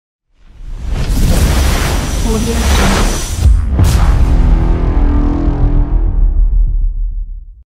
Fire Eye Logo Reveal (Top After Effects Templates)
Fire Eye Logo Reveal After Effects Cs5.5+ template . Project presented at FullHD (1920×1080). This project can help you create very cool animation for your logo. Project very easy and fast modyfy.

Replace the logo with your company logo or just introduce text
Easy to edit logo and text
No plugins
Fast Render
1920×1080 Full HD
Support 24/7